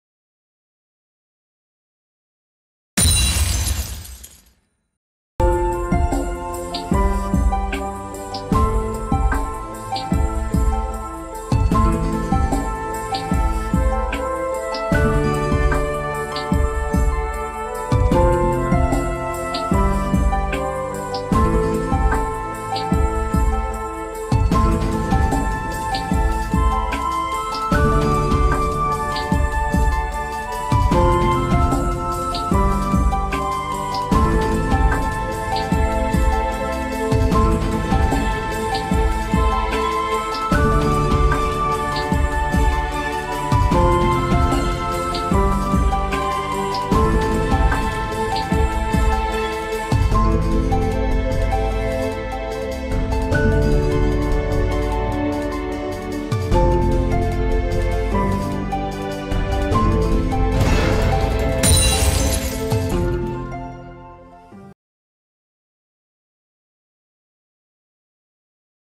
[ Four Gods on Wemix ] - Various Way to Obtain RedGem !
- Hi guys !
- This video from Wemix about obtaining RedGem in Four Gods, the incoming P2E Game.